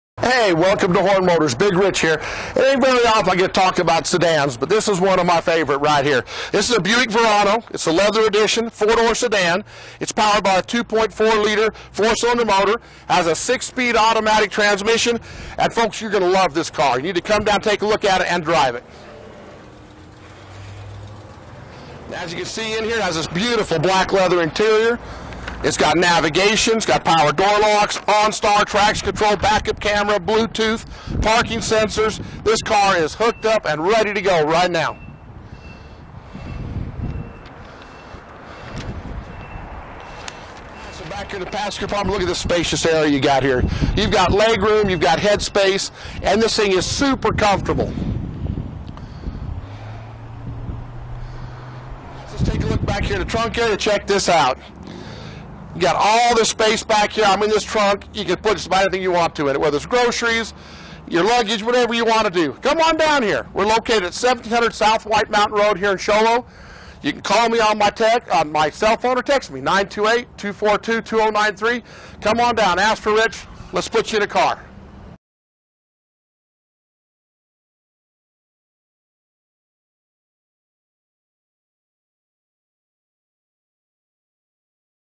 ID-5491-Buick Verano Leather Sedan 4D
2014 Buick Verano Leather Sedan 4D for sale at Horne Motors Show Low, Arizona.